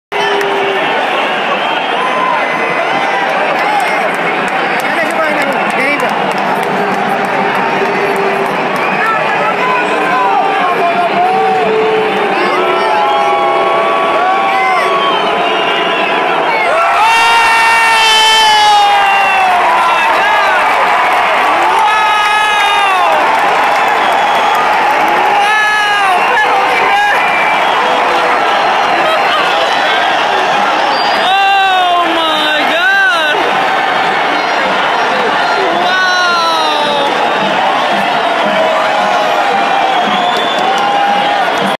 Spain vs El Salvador International Friendly: Fabregas Fail
Only 4 minutes in and Diego Costa wins a penalty. The Legendary Cesc Fabregas steps up to take it. Easy money right?